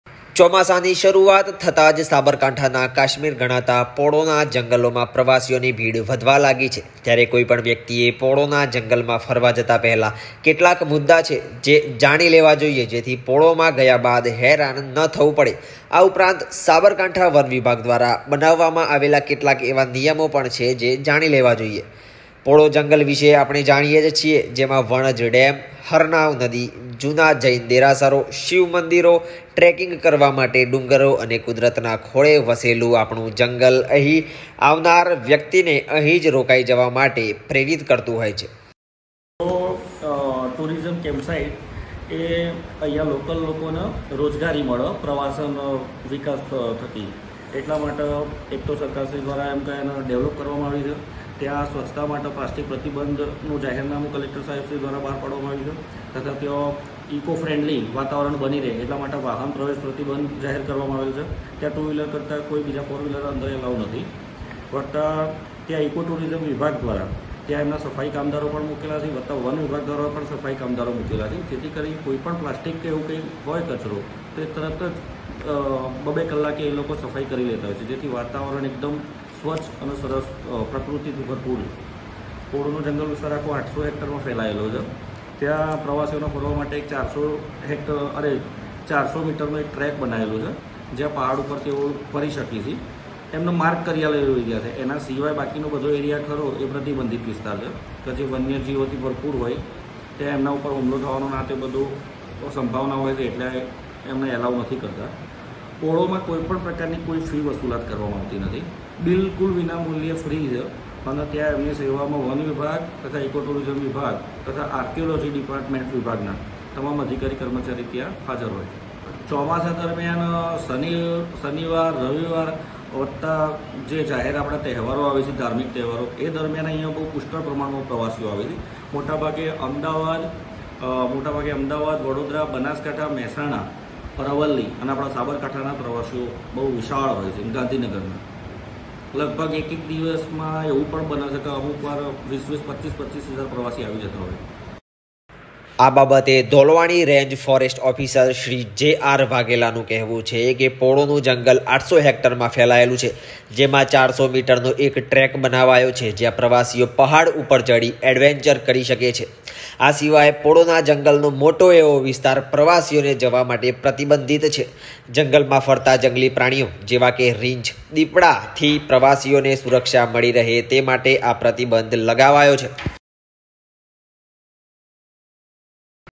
Polo Forest | વિજયનગર પોળો ફોરેસ્ટની આ સાઈટો છે જોવા જેવી, જુઓ Video
Polo Forest | These sites of Vijayanagar Polo Forest are worth seeing, see Video

ચોમાસાની શરૂઆત થતા જ સાબરકાંઠાના કાશ્મીર ગણાતા પોળોના જંગલમાં પ્રવાસીઓની ભીડ વધવા લાગી છે. ત્યારે કોઈ પણ વ્યક્તિએ પોળોના જંગલમાં ફરવા જતા પહેલા કેટલાક મુદ્દા છે જે જાણી લેવા જોઈએ.જેથી પોળોમાં ગયા બાદ હેરાન ન થવું પડે. આ ઉપરાંત સાબરકાંઠા વન વિભાગ દ્વારા બનાવવામાં આવેલા કેટલાક નિયમો પણ જાણી લેવા જોઈએ.